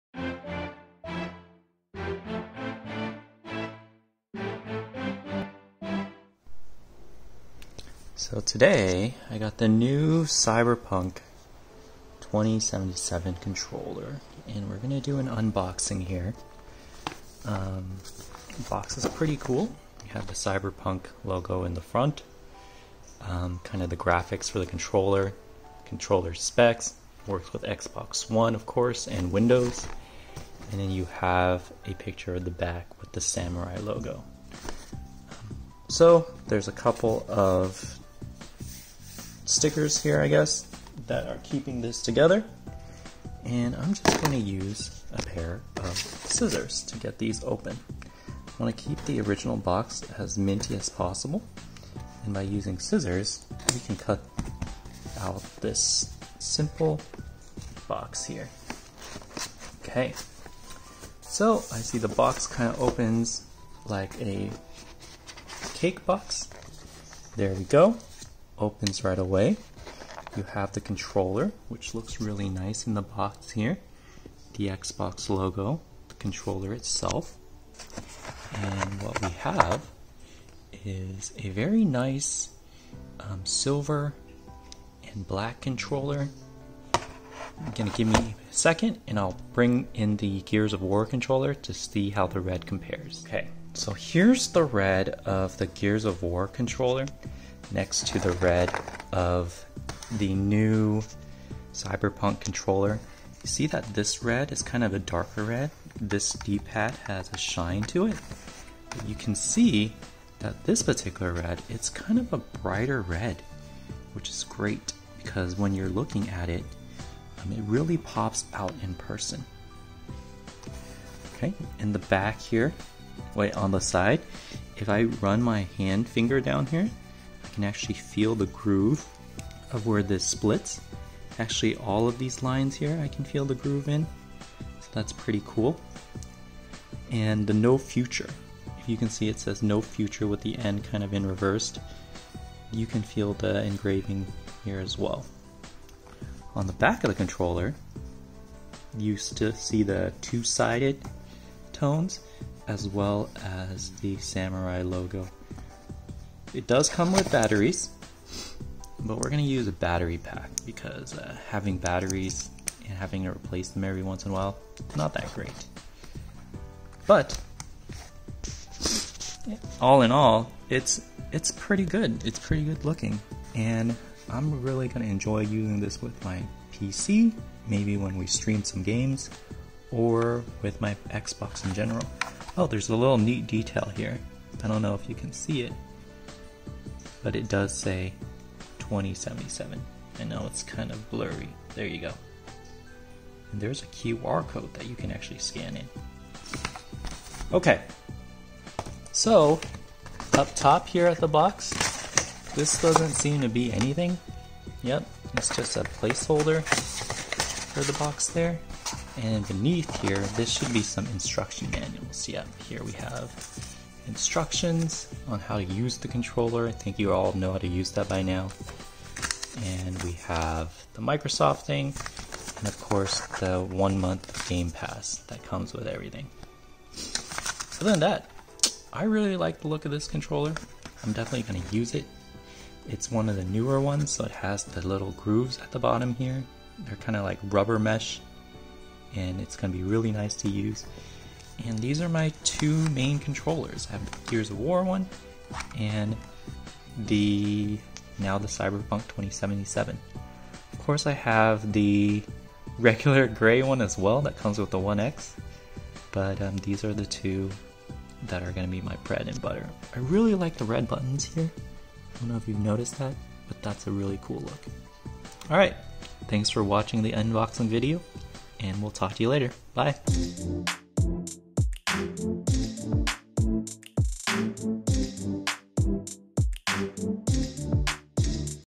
CyberPunk 2077 Controller - Unboxing & Comparison!
Just received my cyberpunk2077 xbox controller and wanted to share it with you guys!